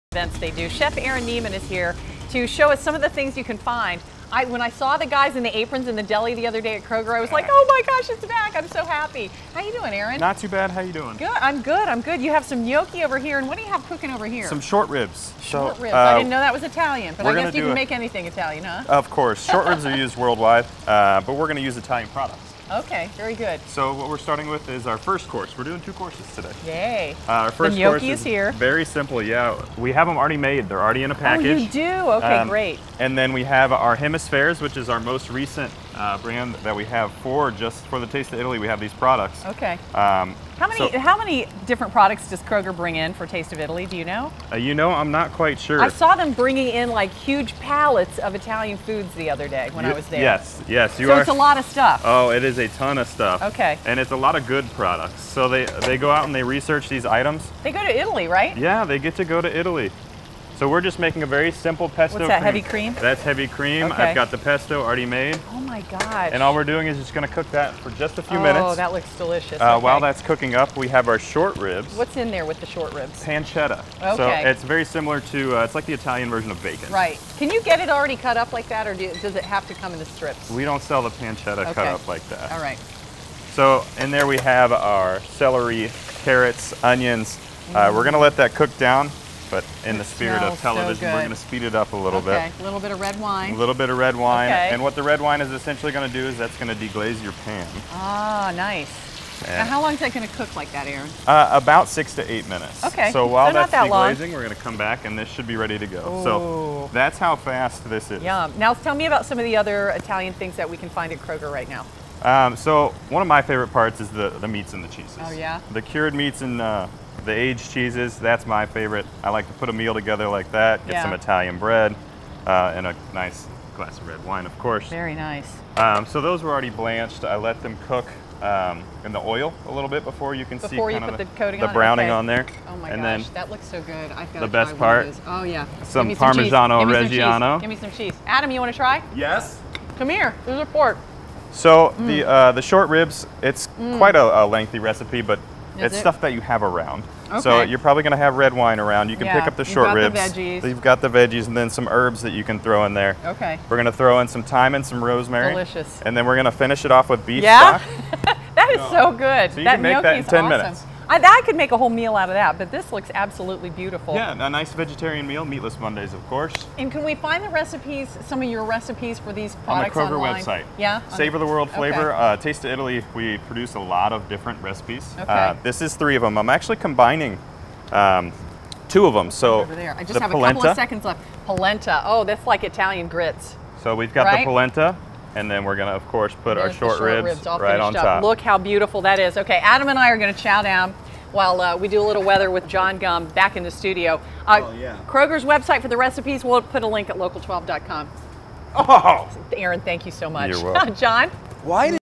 Experience a Taste of Italy with this dish from Kroger chef
CINCINNATI (WKRC) - Taste of Italy is back at Kroger. Chef Aaron Nieman from Kroger shows how to whip up a gnocchi dish.